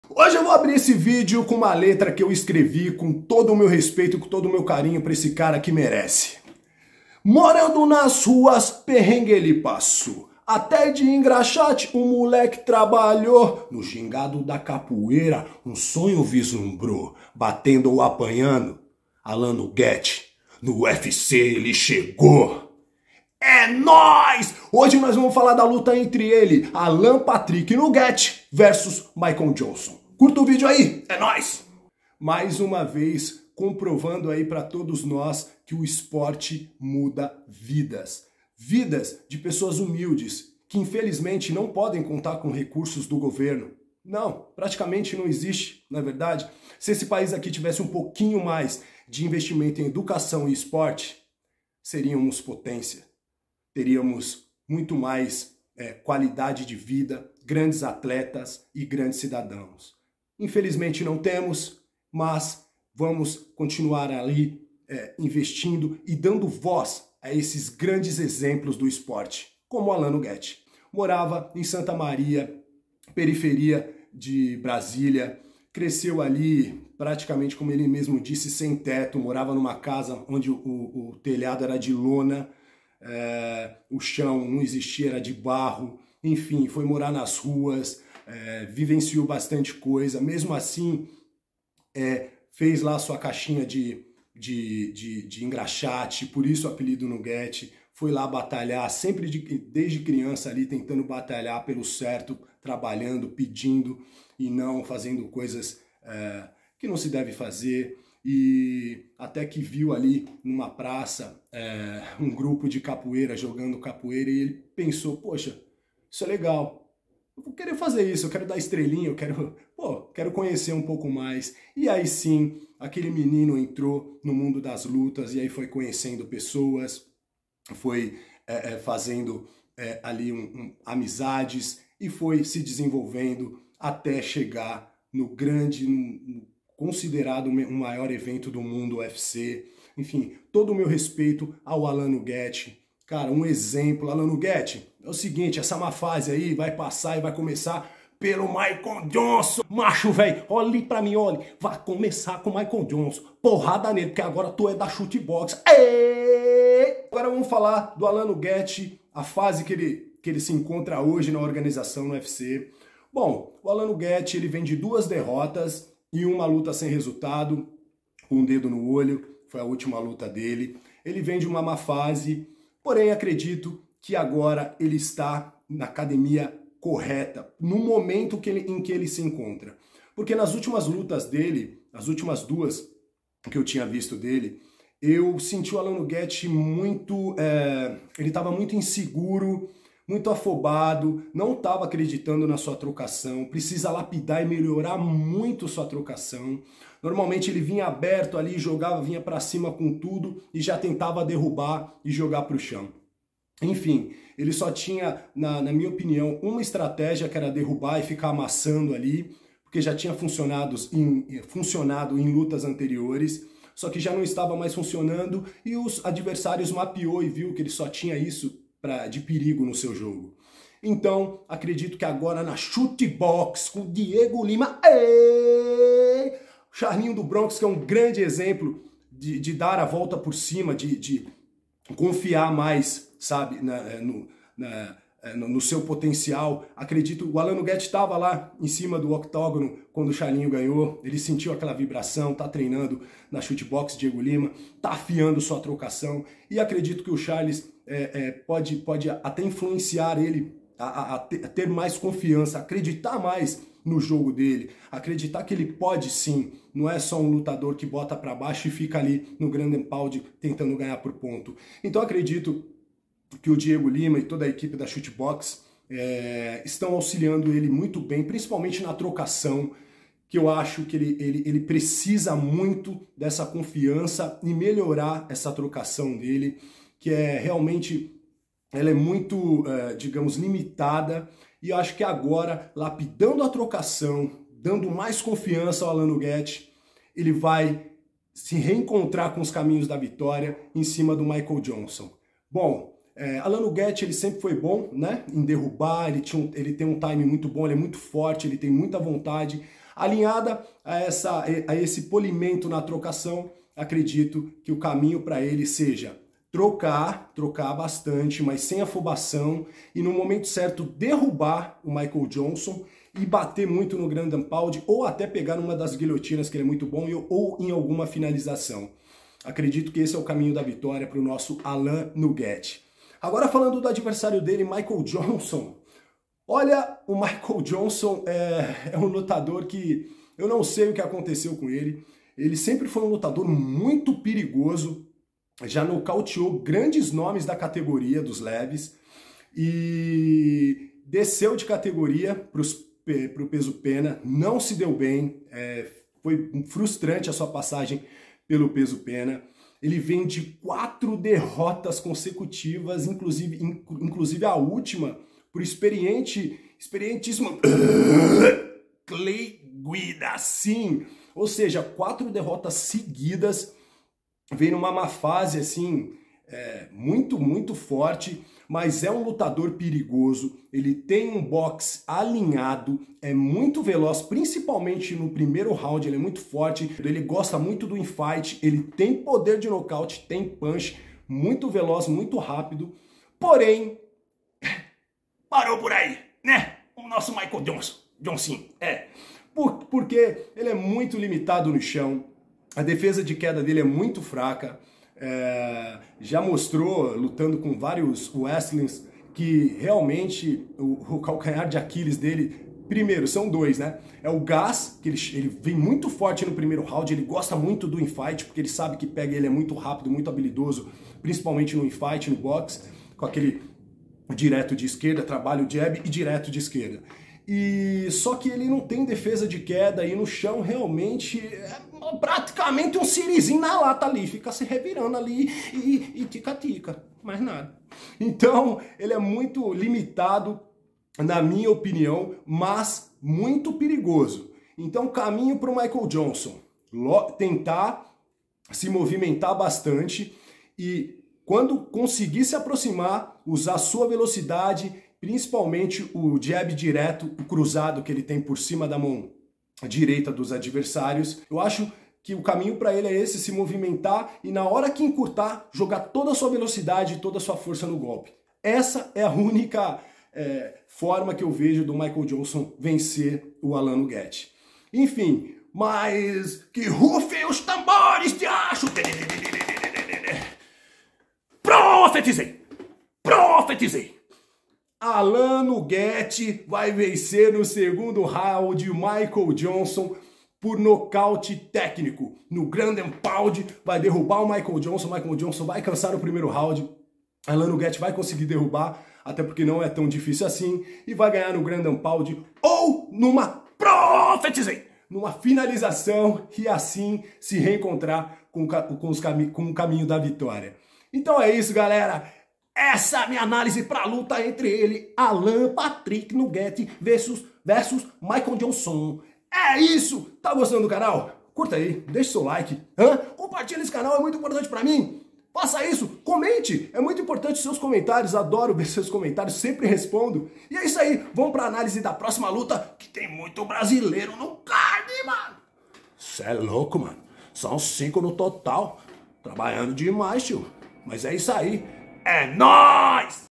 Alan Patrick Nuguette vs Michael Johnson. PALPITE UFC FIGHT NIGHT
Alan Nuguette vs Michael Johnson.
UFC FIGHT NIGHT
ANÁLISE E PALPITE DESSA LUTA!